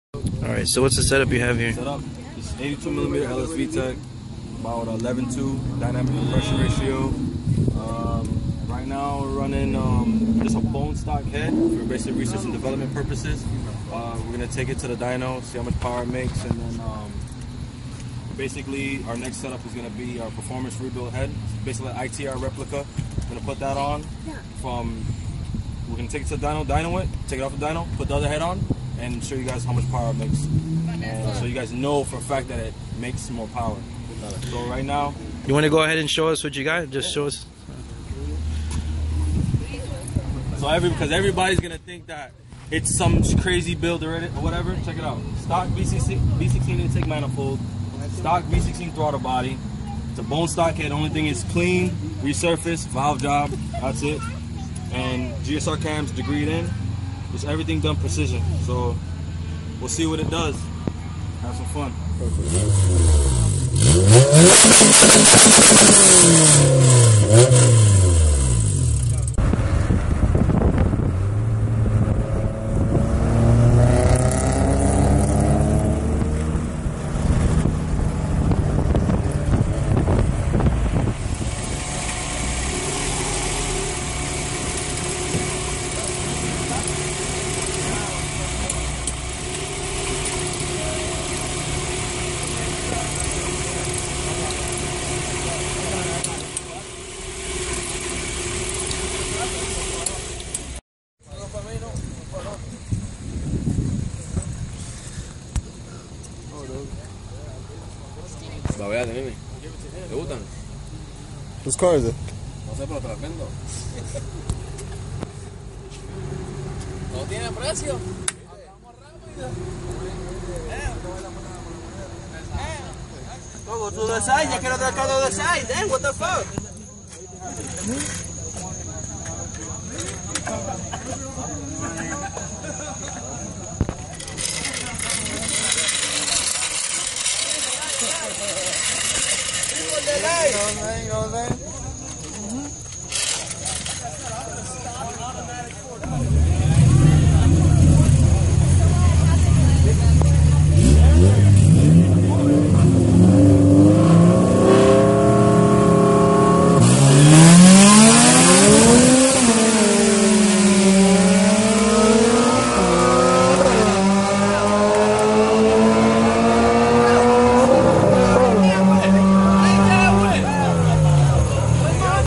Street image TV BMRENGINEERING Allmotor b-series vs Allmotor kseries
This is the start of our research and development at BMRengineering our setup
BLOCK
82mm ls vtec
ctr pistons
stock rods ( shop peened )
micro polished crank
race blue printed, balanced and assembled in house
HEAD
OEM rebuild b16 head ( starting price $200 )
- disassembled
- Cleaned
- valve job
- Valve seals
- resurfaced
Super tech dual valve springs and retainers
stock valves
stock intake manifold
stock b16 throttle body
550cc rc injectors
f1 stage 3 clutch
xsi b16 transmission with 3rd and 4th carbon synchros welded diff
conservative street tune on crome gold in house on pump 93 entill dyno time

the race was against a stock k20 with an rbc full bolt ons big tube headers street tuned on e85